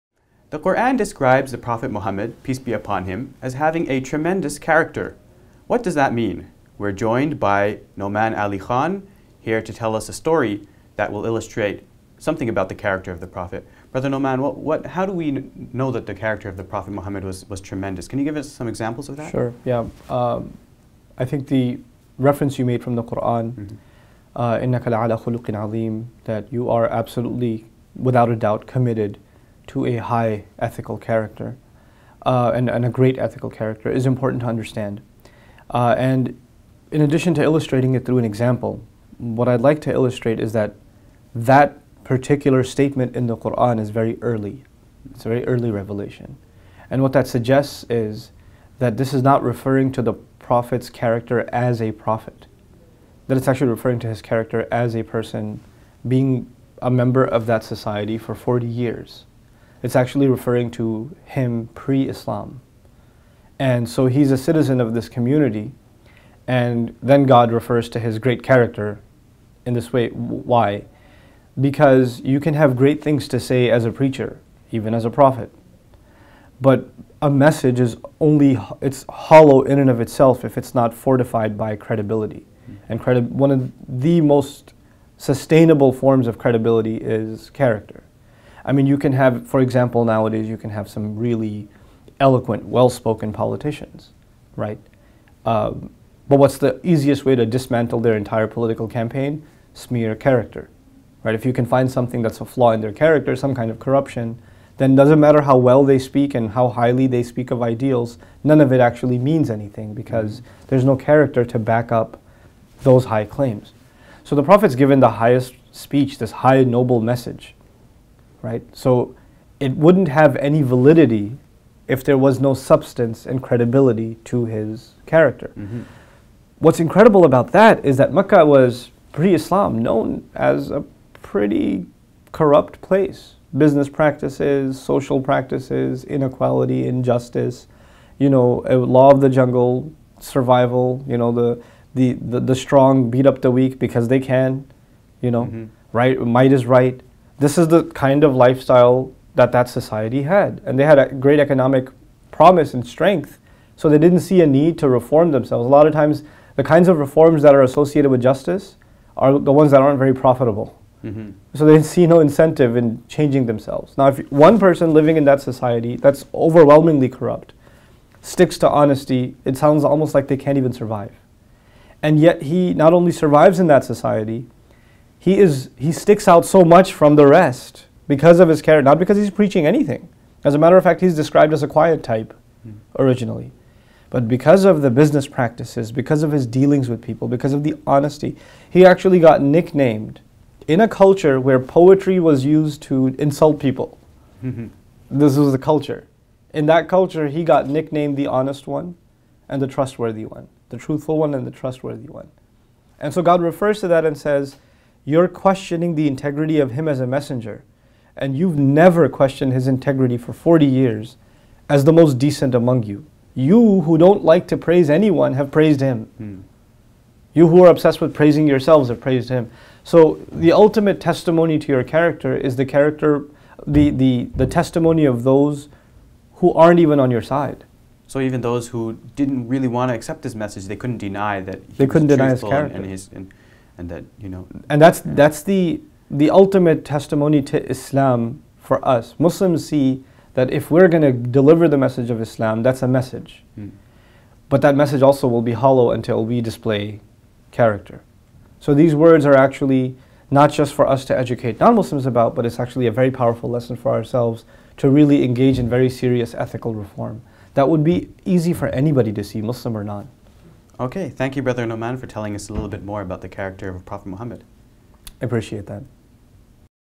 Uniqueness of Verse on Tremendous Character of Prophet (pbuh) | Nouman Ali Khan
Nouman Ali Khan shares with us what is special about God referring to the Prophet (pbuh) as having a 'tremendous character'.